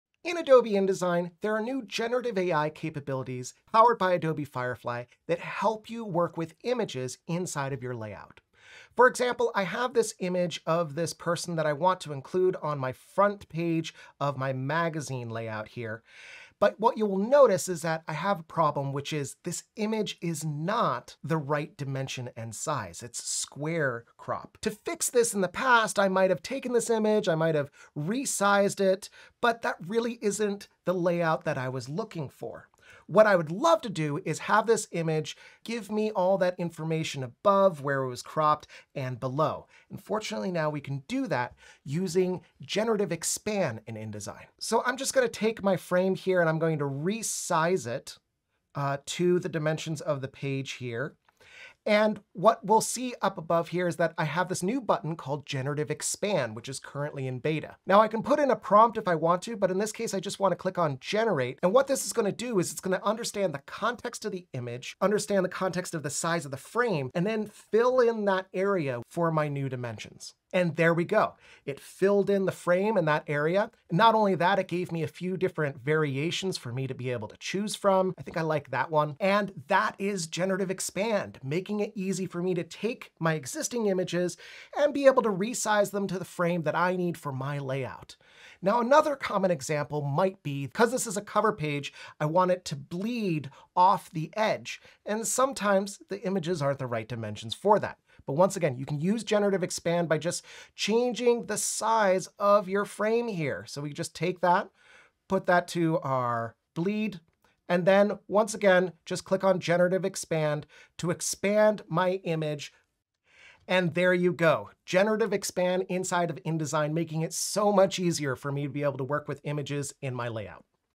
Expand images in Adobe InDesign and Firefly
See how you can use the new Generative Expand (Beta) feature in Adobe InDesign to take images and resize them to fit the frames you need in your layout.